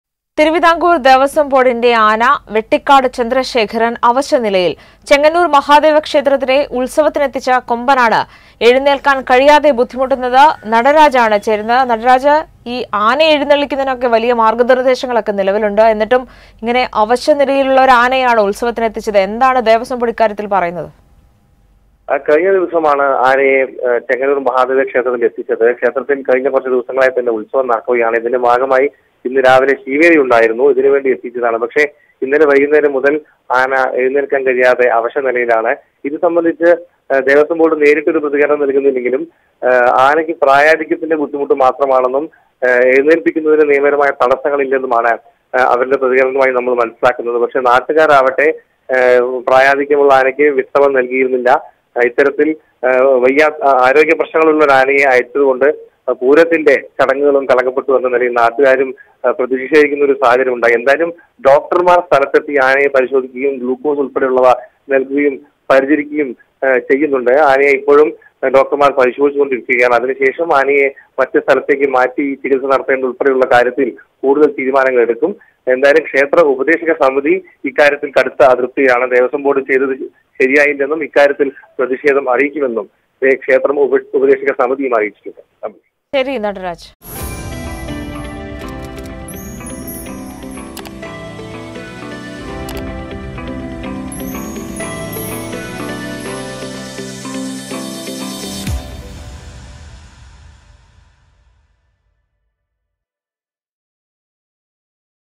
എഴുന്നള്ളിപ്പിനെത്തിച്ച ആന എഴുന്നേൽക്കാൻ പോലുമാവാതെ അവശനിലയിൽ | Vettikkadu Chandrasekharan
എഴുന്നള്ളിപ്പിനെത്തിച്ച ആന എഴുന്നേൽക്കാൻ പോലുമാവാതെ അവശനിലയിൽ; വീണുകിടക്കുന്നത് വെട്ടിക്കാട് ചന്ദ്രശേഖരൻ